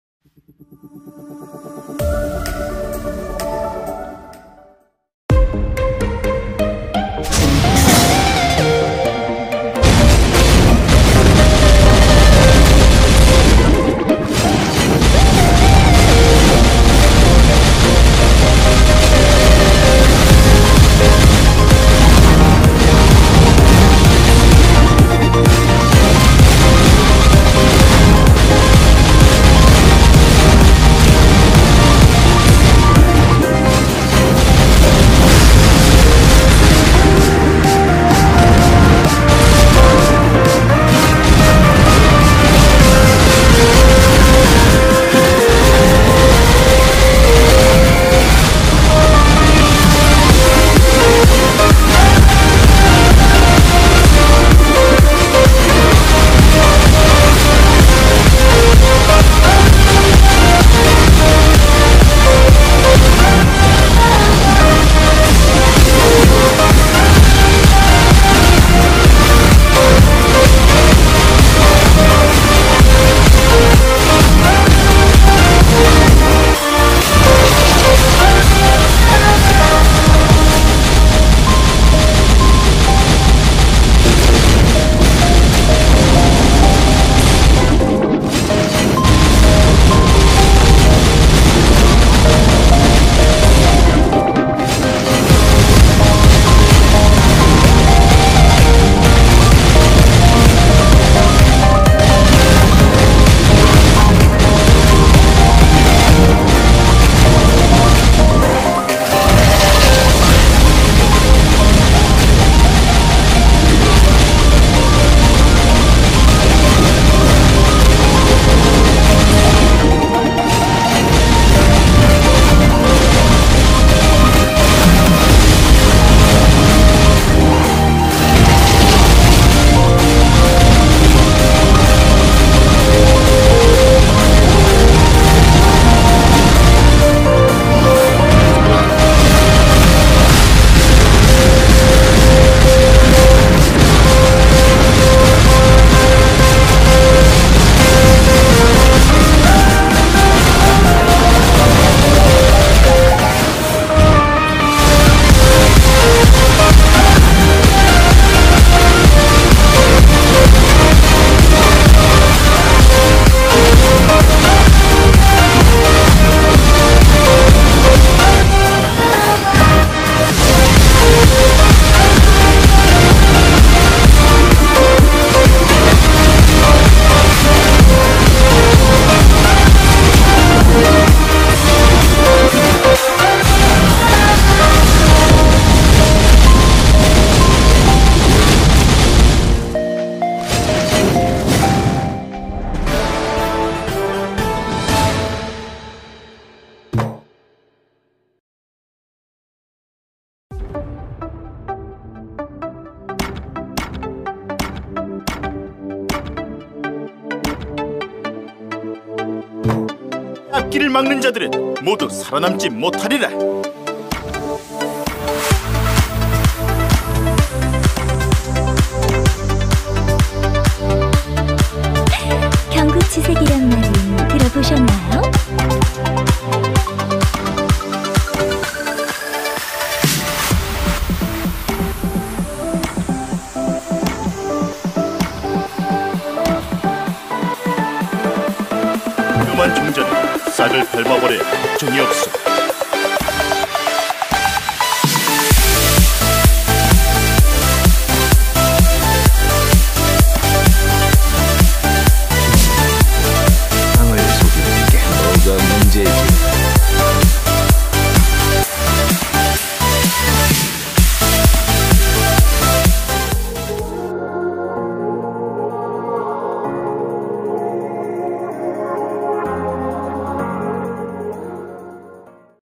[토벌전] - 여포⚔ 공략 #8 [bloodyTV][블러디TV]
00:00 오프닝
00:05 Boss 여포
03:20 스팩 확인(손견/조비/산초/육손/방통)

패턴 3가지
※ 폭탄병을 틈나는대로 제거, 모아둔 필살기를 주력 무장에서 사용
1. 폭죽
2. 돌진(약간 빗겨 있으세요, 절대 방어 유효함)
3. 기절(필살기 추천)